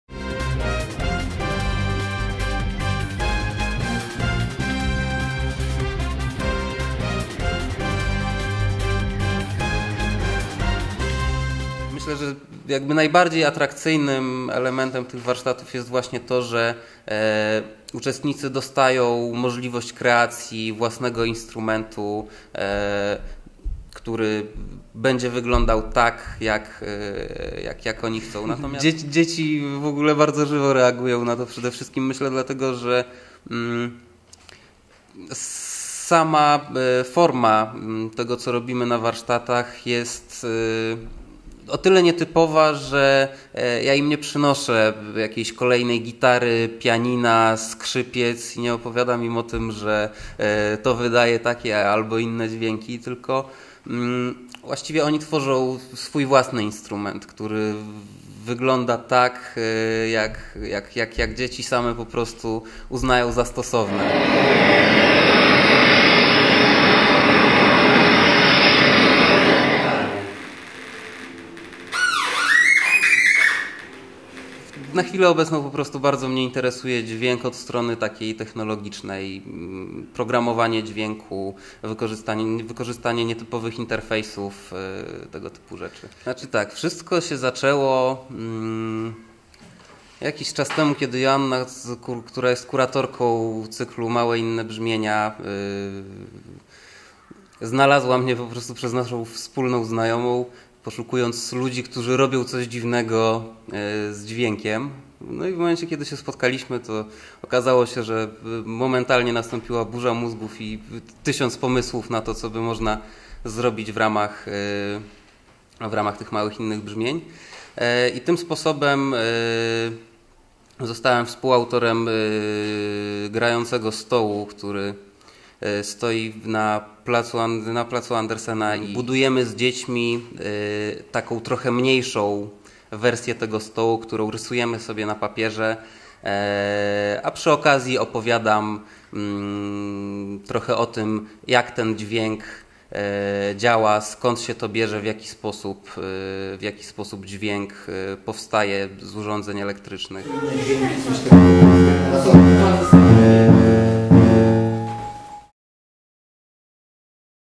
Festiwal Inne Brzmienia, warsztaty - wywiad z Maciejem Połynko, Lublin 2014 - TOtv.pl
Odwiedzając kolejne odsłony Festiwalu Inne Brzmienia zajrzeliśmy również na warsztaty z tworzenia eksperymentalnych instrumentów muzycznych - opowiada nam o tym Maciej Połynko.